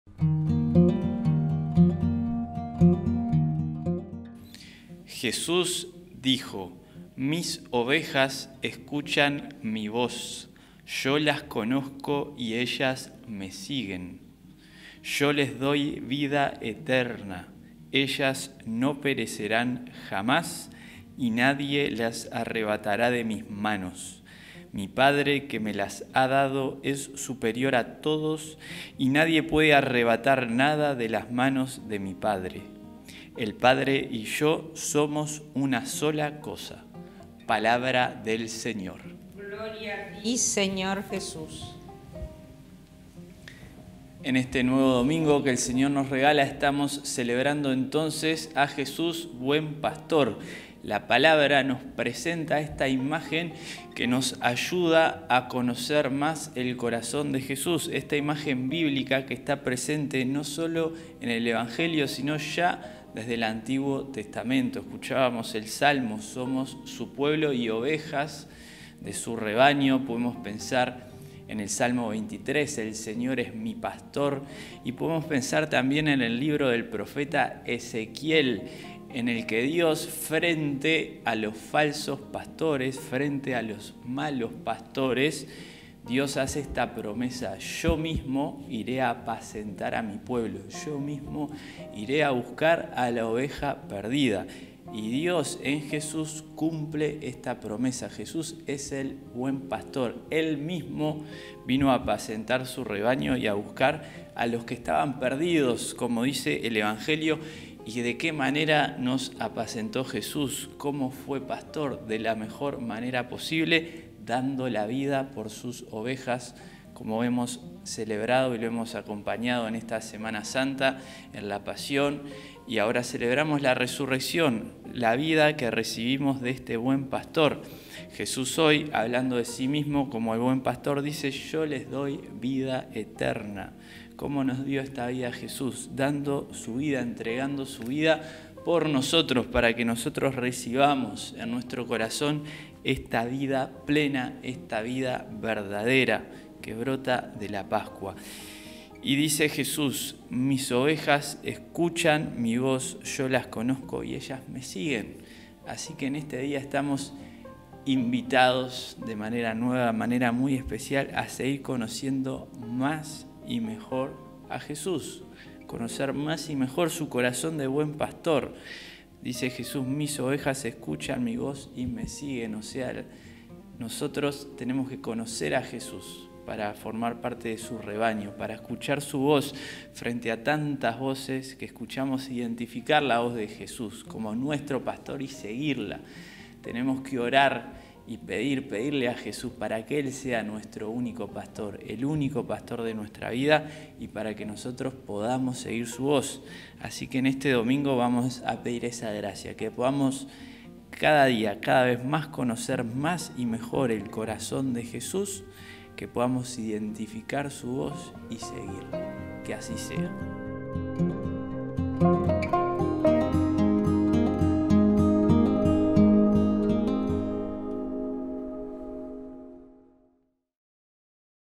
Evangelio del día - Juan 10, 27-30 | 08 de Mayo 2022
Te compartimos el Evangelio y la homilía del Domingo 08 de Mayo del 2022 a cargo del P. Luis Ferrés

Evangelio de nuestro Señor Jesucristo según san Juan 10, 27-30

En aquel tiempo, Jesús dijo:
      «Mis ovejas escuchan mi voz, yo las conozco y ellas me siguen. Yo les doy Vida eterna: ellas no perecerán jamás y nadie las arrebatará de mis manos. Mi Padre, que me las ha dado, es superior a todos y nadie puede arrebatar nada de las manos de mi Padre. El Padre y yo somos una sola cosa.»